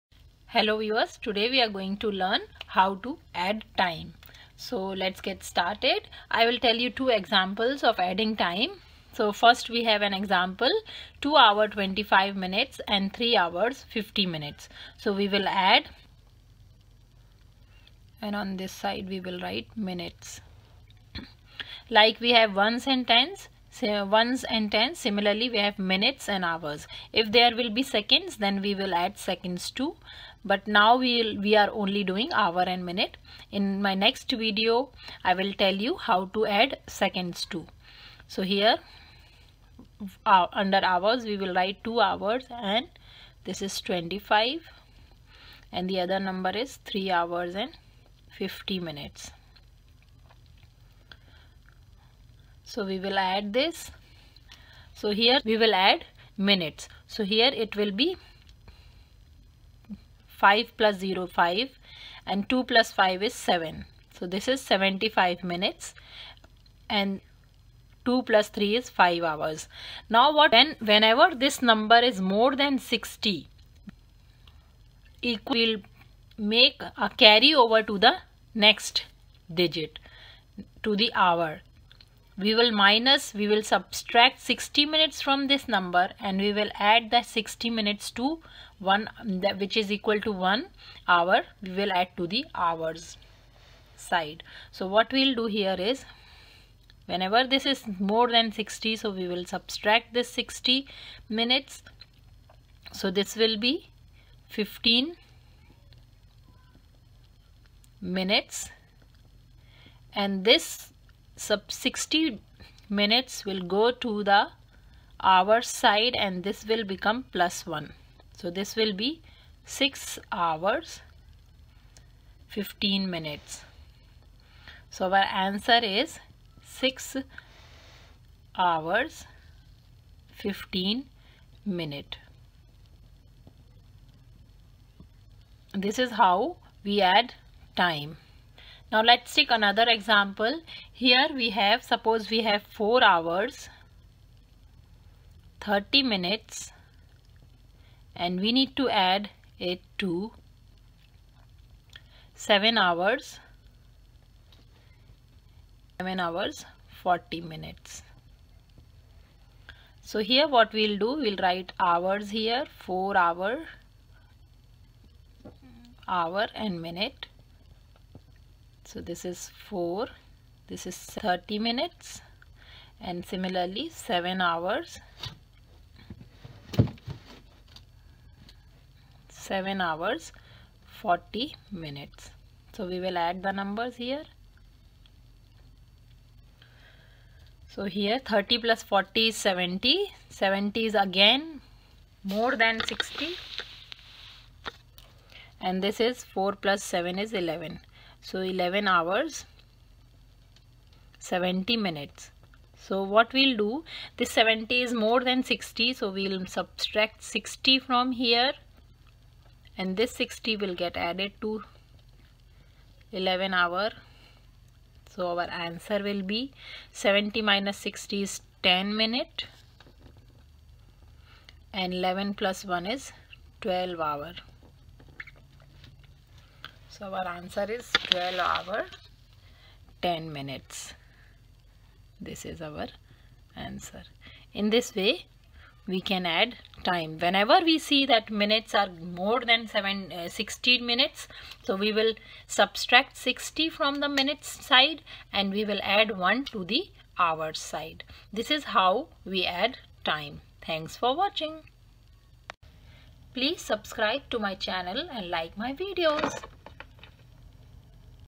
ADDING TIME (HOURS AND MINUTES)
In this video you will learn how to add time (hours and minutes)